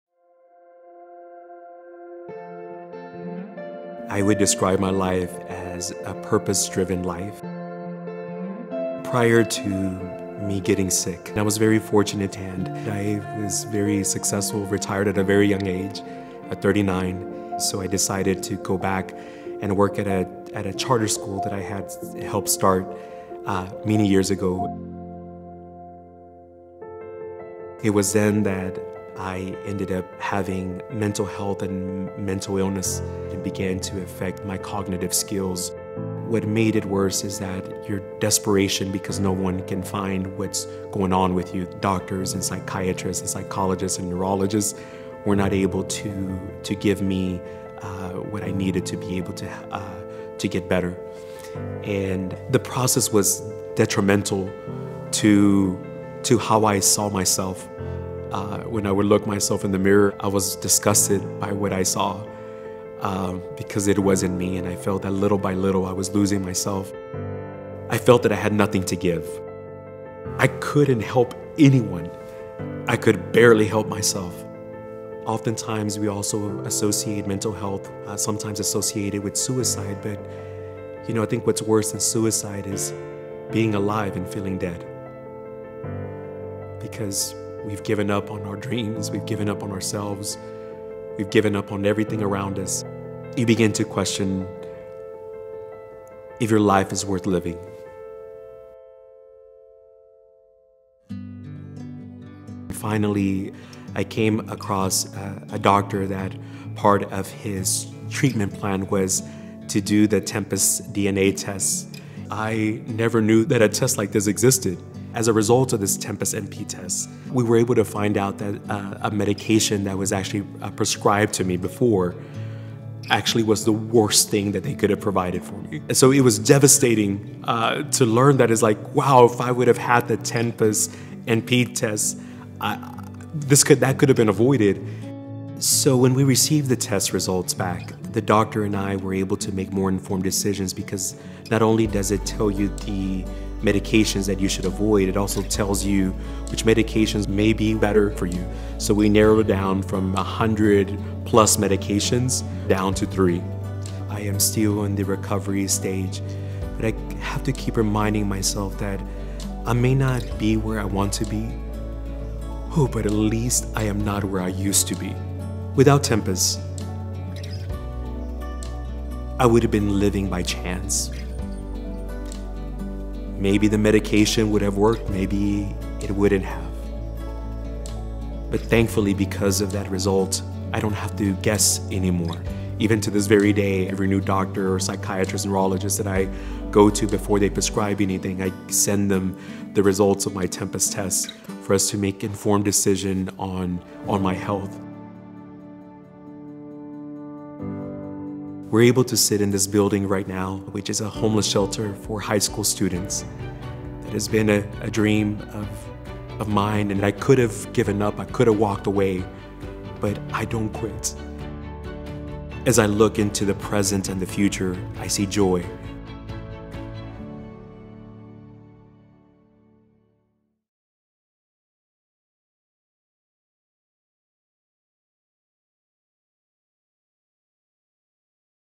Patient Story: Mateo | Tempus
Mateo shares his personal story of battling depression and how the Tempus nP pharmacogenomic test helped inform a different medication selection that positively impacted his treatment.